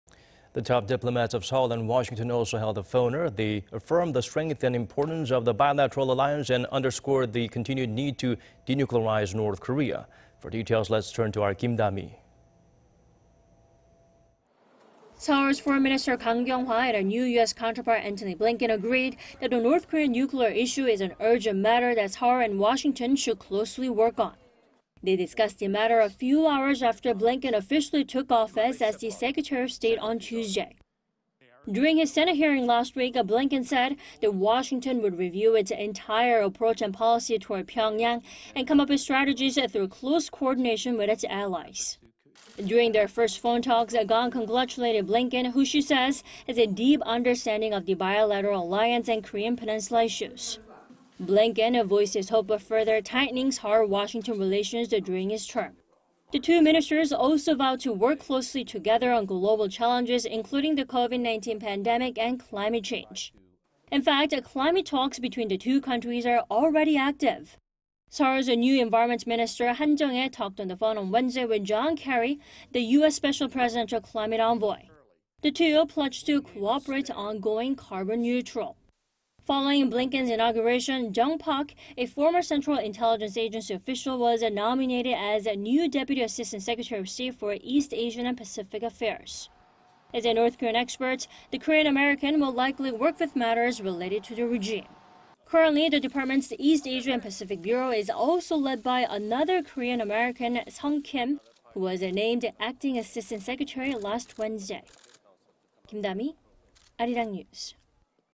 Kang, Blinken agree in phone talks that N. Korea nuclear issue is urgent matter to resolve
강경화, 블링컨 통화 "북핵문제 시급히 다뤄야할 문제"

Top diplomats of Seoul and Washington also held a phoner.
They affirmed the strength and importance of the bilateral alliance and underscored the continued need to denuclearize North Korea.
For details let's turn to our Kim Dami
Seoul's foreign minister Kang Kyung-wha and her new U.S. counterpart Antony Blinken agreed that the North Korean nuclear issue is an urgent matter that Seoul and Washington should closely work on.
They discussed the matter a few hours after Blinken officially took office as the Secretary of State on Tuesday.
During his Senate hearing last week, Blinken said that Washington would review its entire approach and policy toward Pyeongyang and come up with strategies through close coordination with its allies.
During their first phone talks, Kang congratulated Blinken, who she says has a deep understanding of the bilateral alliance and Korean Peninsula issues.
Blinken voiced his hope of further tightening Seoul-Washington relations during his term.
The two ministers also vowed to work closely together on global challenges including the COVID-19 pandemic and climate change.
In fact, climate talks between the two countries are already active.
Seoul's new environment minister Han Jeong-ae talked on the phone on Wednesday with John Kerry, the U.S. special presidential climate envoy. The two pledged to cooperate on going carbon neutral.
Following Blinken's inauguration, Jung Pak, a former Central Intelligence Agency official, was nominated as new Deputy Assistant Secretary of State for East Asian and Pacific affairs.
As a North Korean expert, the Korean-American will likely work with matters related to the regime.
Currently, the department's East Asia and Pacific bureau is also led by another Korean-American, Sung Kim, who was named acting assistant secretary last Wednesday.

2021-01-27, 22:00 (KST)